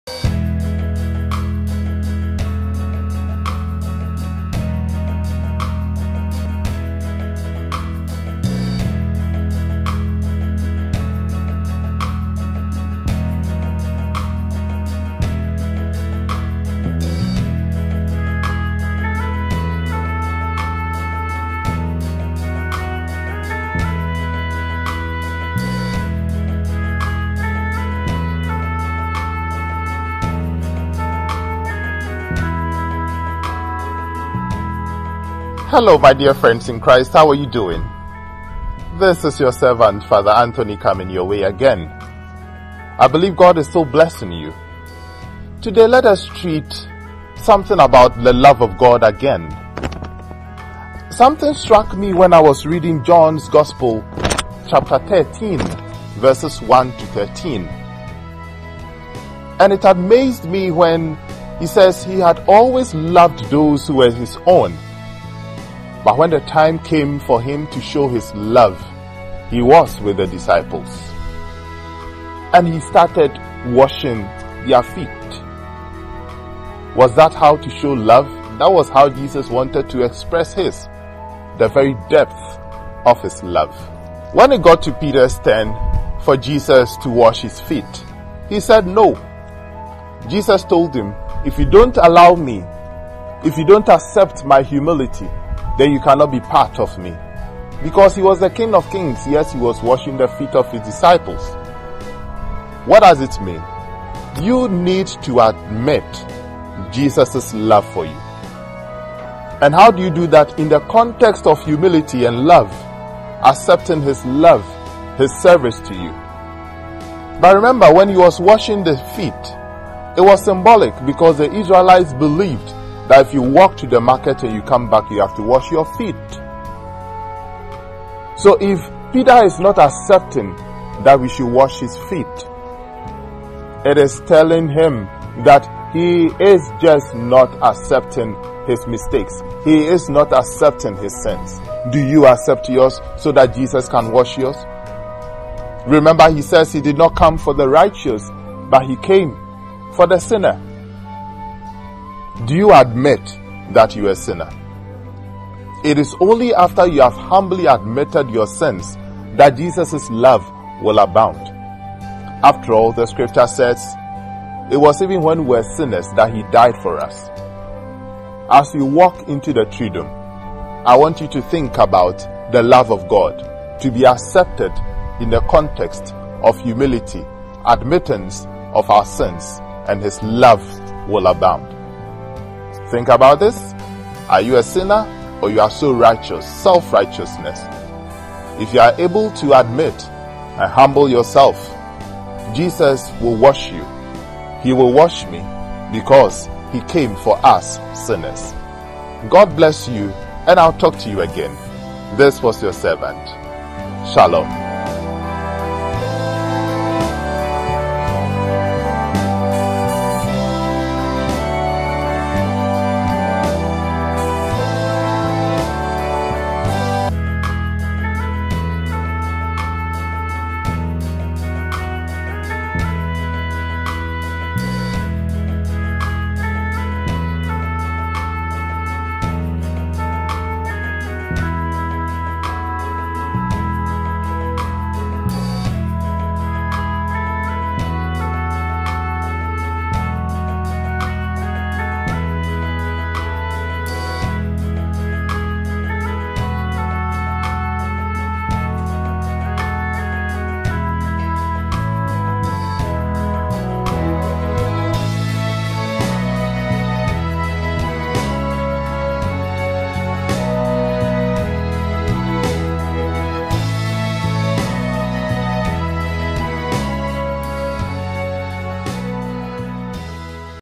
HUMILITY vrs LOVE: By Rev. Fr. A. A. Amponsah (Cath. Archdiocese of Kumasi)
this is a reflection on how to get the best out of the love of Jesus.  A reflection on the LAST SUPPER (HOLY THURSDAY)